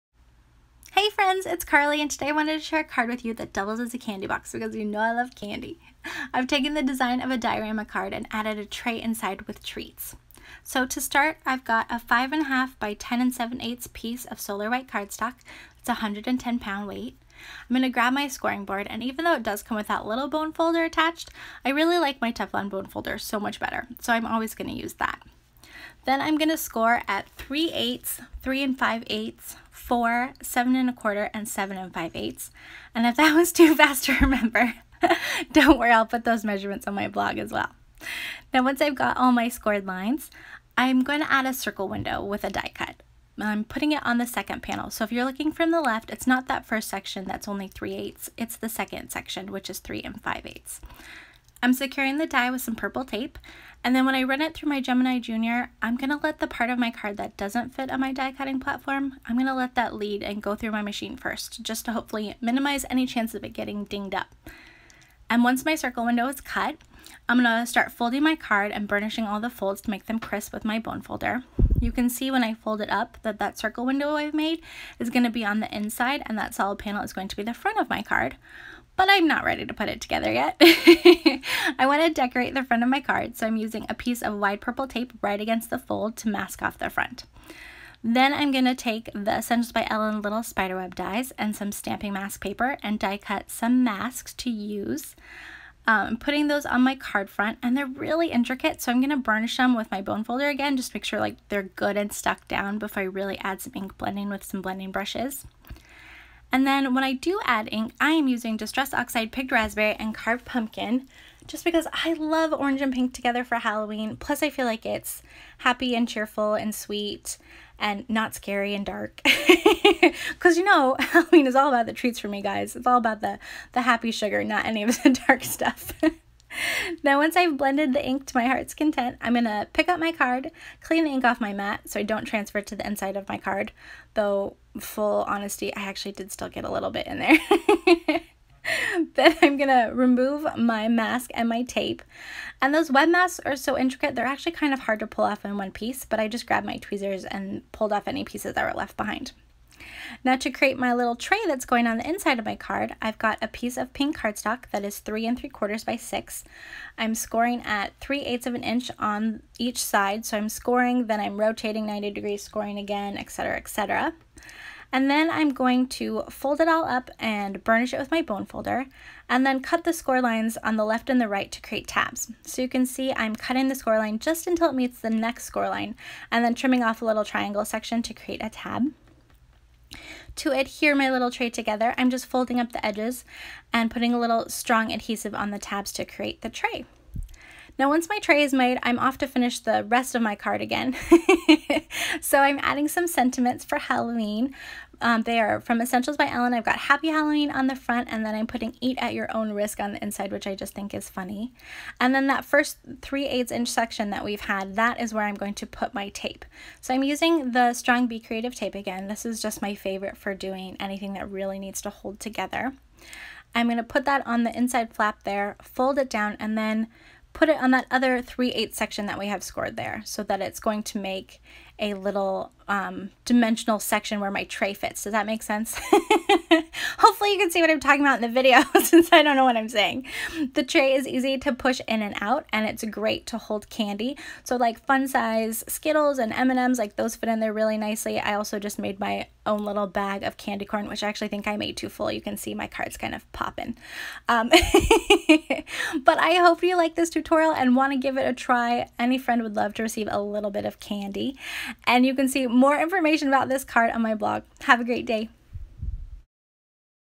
DIY Halloween Treat Card
This little treat box is easy to assemble with just a few cardmaking supplies like a scoring board, a bone folder, cardstock & tape!

The Last Bite by Julie Ebersole, Essentials By Ellen Clear Stamps
Solar White Heavyweight 110 lb - 25 pk, Neenah Classic Crest Cardstock
Life Changing Blender Brush - 4 Piece Broad, Picket Fence Studios (LCB-4)